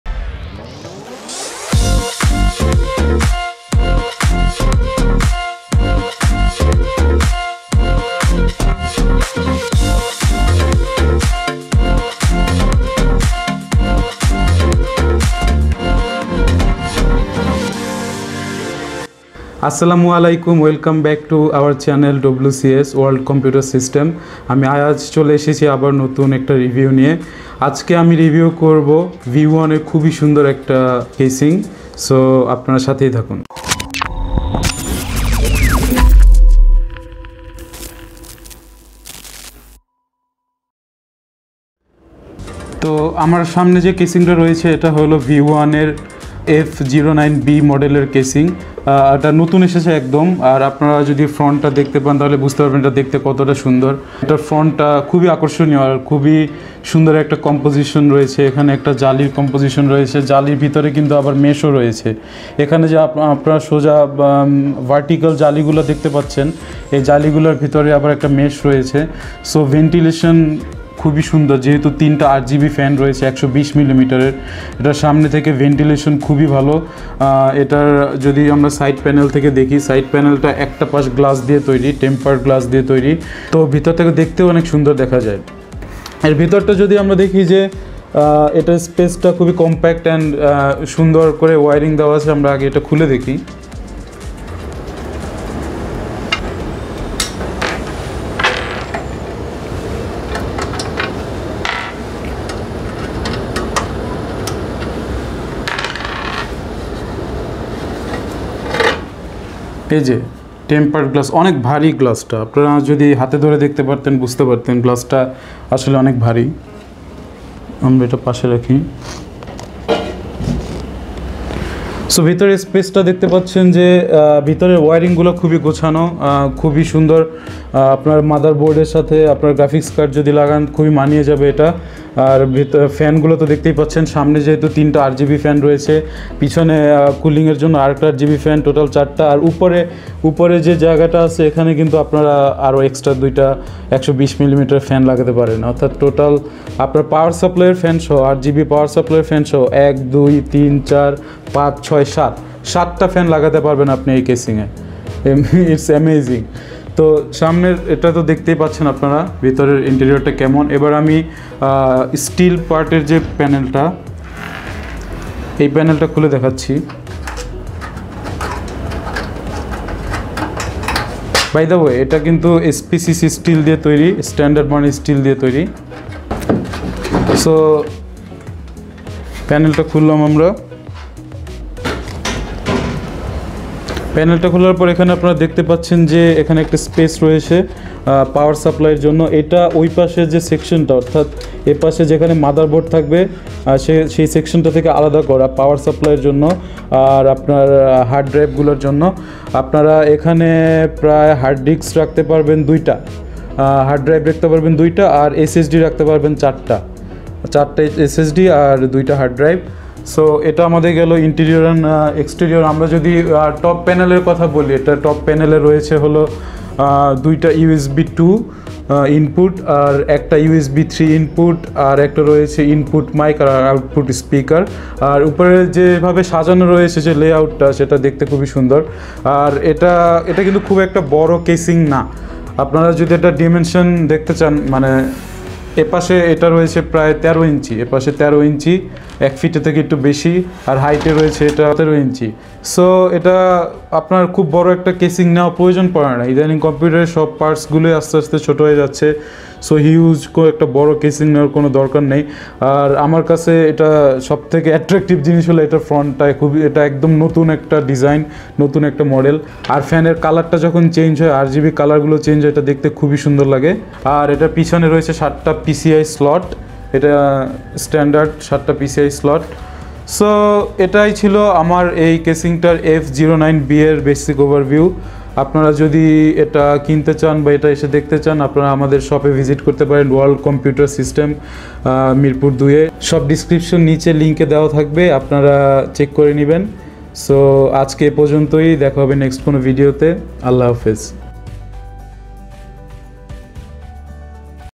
View One F09B Gaming Casing Review/বাজেটের মধ্যে সেরা গেমিং কেসিং
View One F09B Gaming Casing Review

Price: 3900 Taka BDT

Features
Model VF09
Case Type Mid Tower
RGB Cooling Fans
Window Side Panel Tempared Glass
Magnetic Dustproof Net
Front Panel Meash Grill

World Computer System
Level-6 (5th Floor)
Shop No - 626, 638, 640
Mirpur Shopping Center, Mirpur-2
Dhaka -1216